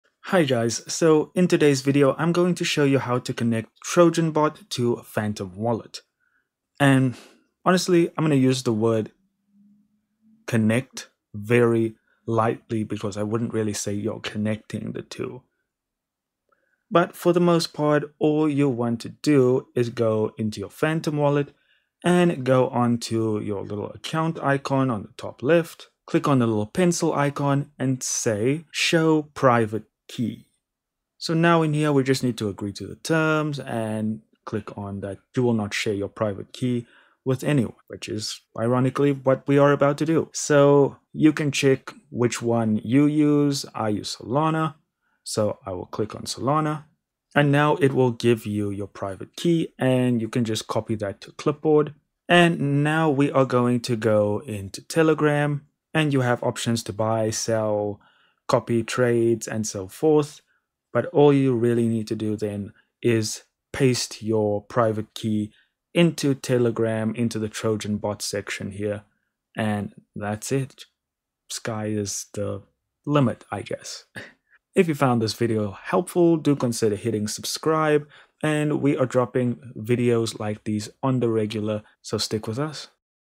How To Connect Trojan Bot To Phantom Wallet - Complete Guide
Learn how to connect Trojan Bot to your Phantom Wallet in this easy-to-follow guide! Whether you're setting up your Phantom Wallet for the first time or linking it with the Trojan Bot, we've got you covered. Discover step-by-step instructions to ensure a seamless connection between Trojan Bot and Phantom Wallet. Perfect for beginners and experts alike who want to get the most out of their Trojan Bot and Phantom Wallet experience. Watch now and start connecting with confidence.